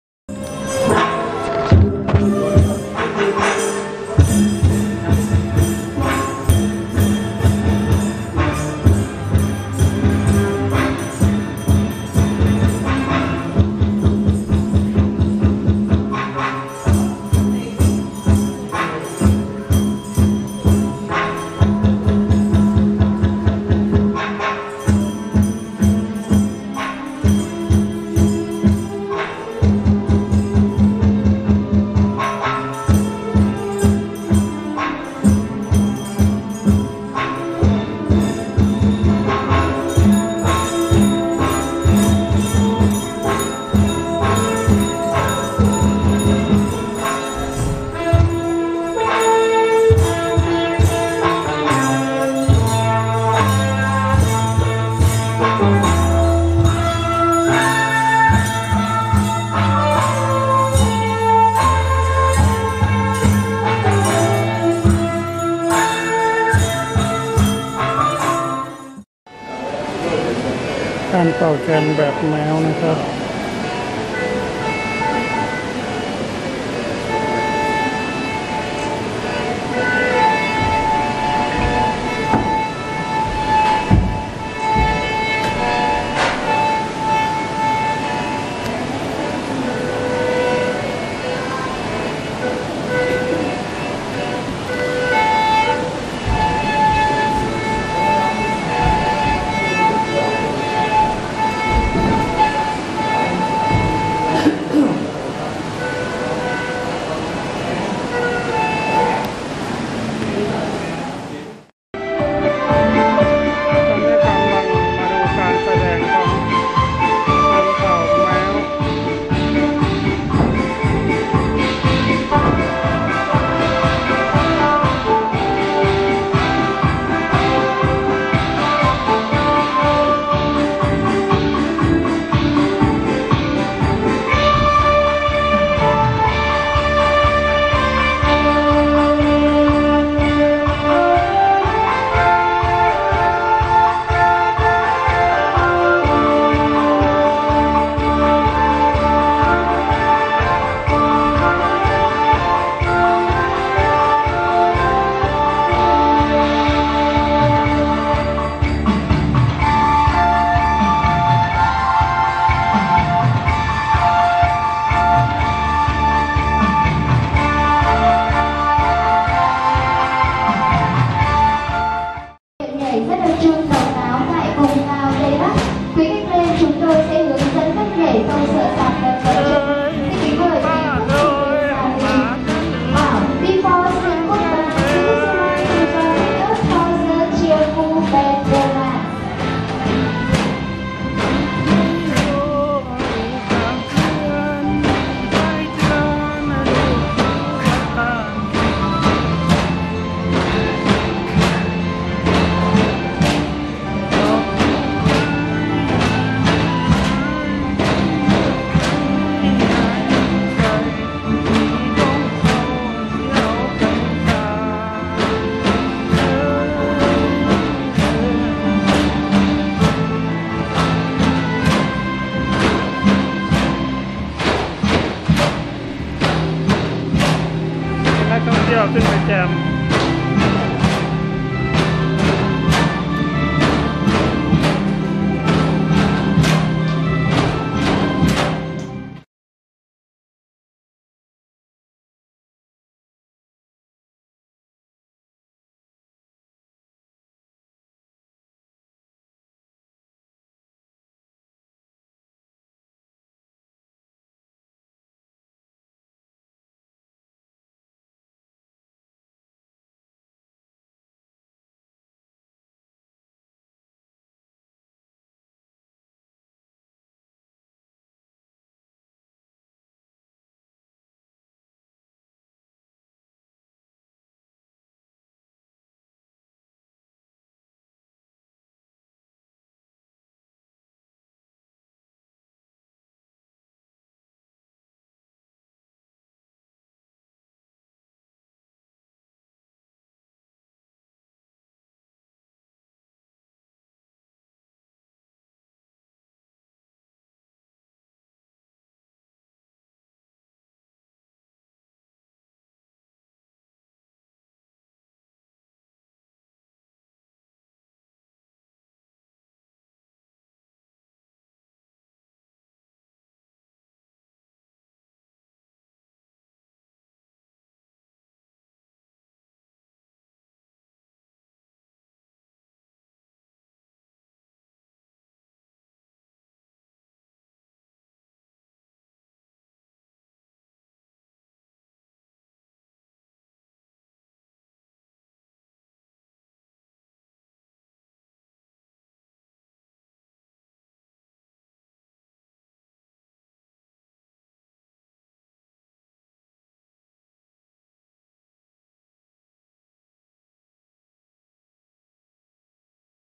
การแสดงศิลปวัฒนธรรม 5 ชนเผ่า ที่เมืองซาปา เผ่าดั้งเดิมก่อนที่รัฐบาลจะเข้าไปพัฒนา
การแสดงดั้งเดิมของคนไทยเขา ซึ่ง ที่นี่เป็นถิ่นกำเนิดของชนชาติไต และบางหมู่บ้านพูดภาษาลาวโซ่งหรือไทยใหญ่ผสม ยังสามารถพูดภาษาลาวเหนือคุยกันได้